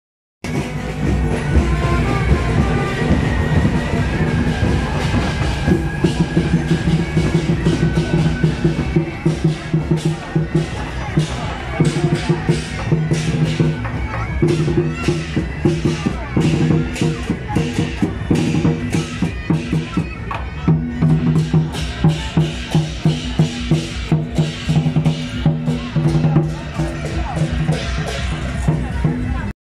【新店聖和舘-醒獅團】~110木柵聖母宮天上聖母南巡進香回駕遶境
新店聖和舘-醒獅團